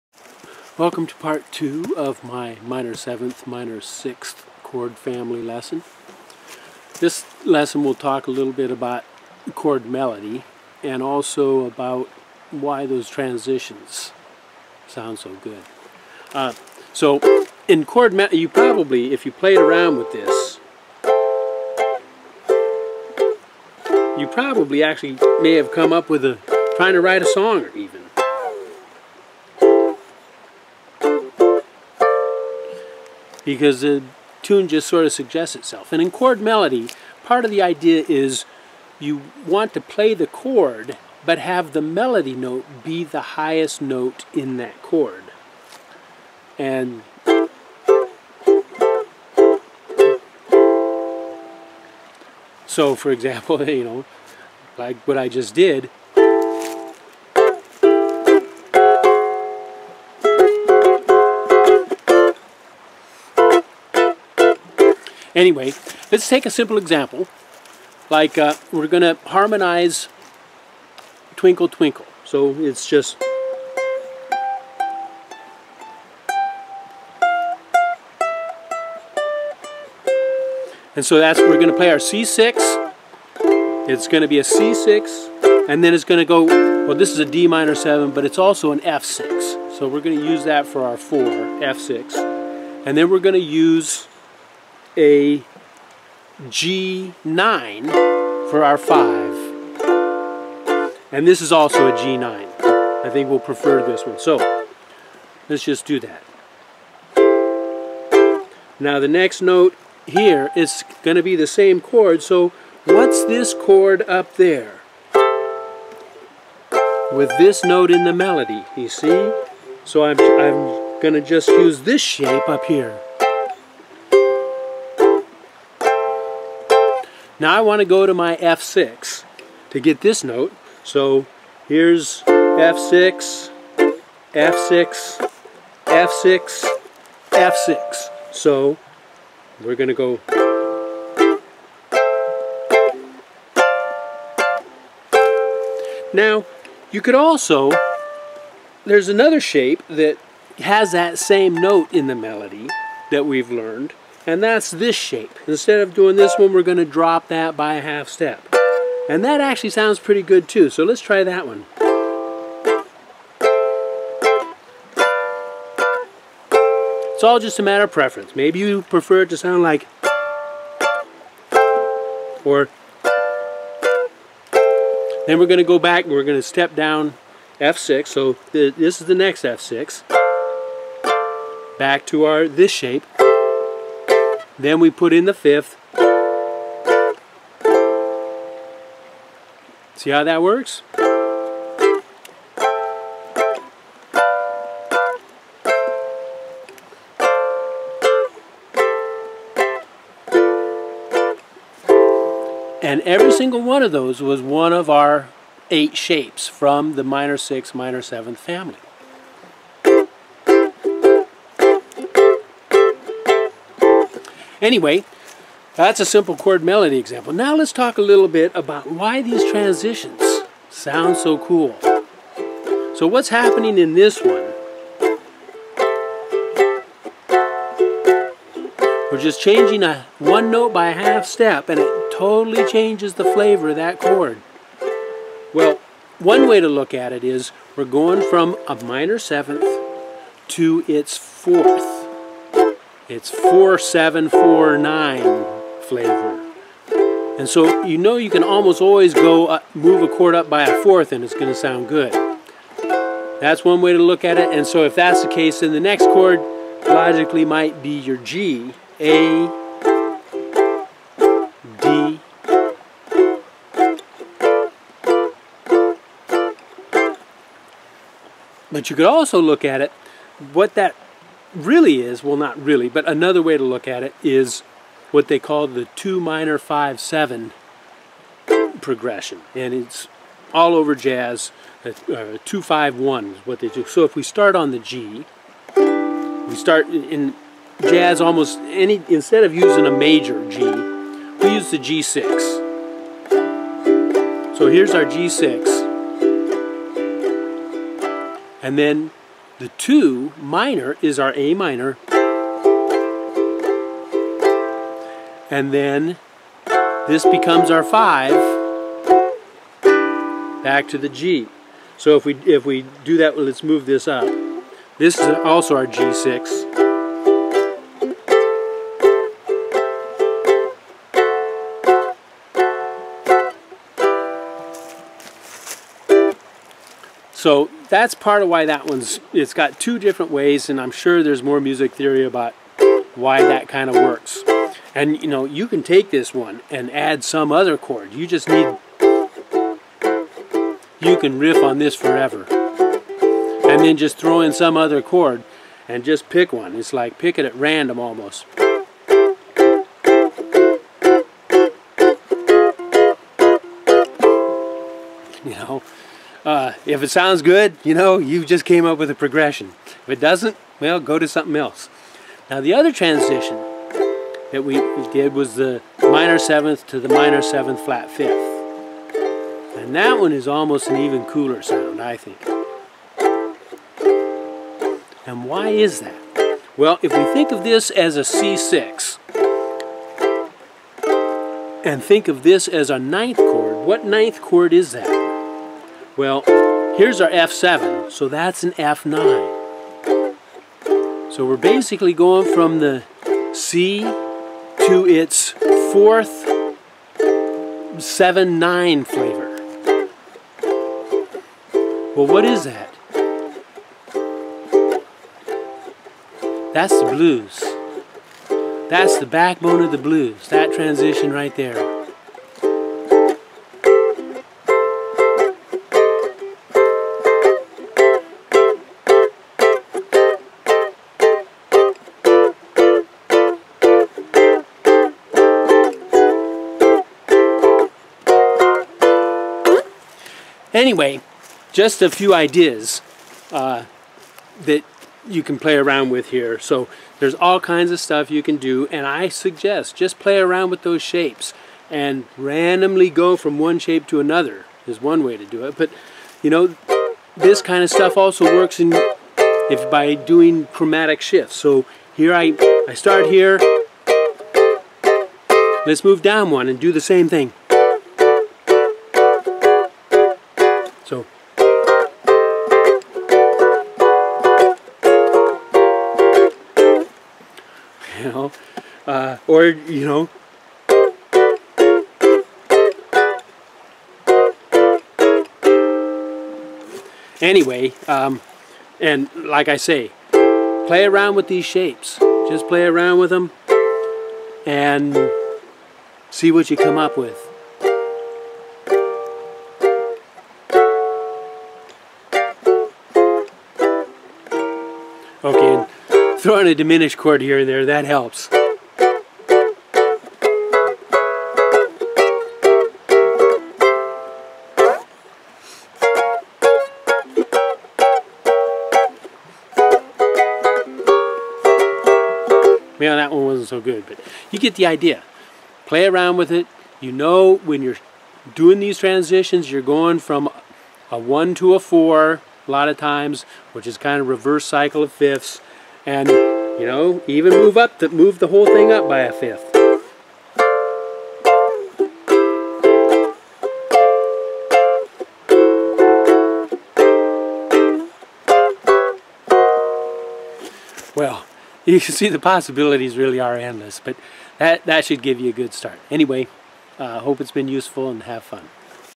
uke lesson - moving up the neck part 2 - m7 m6 chords
How these chords can be useful for chord-melody arrangements and for coming up with riffs and progressions for songwriting.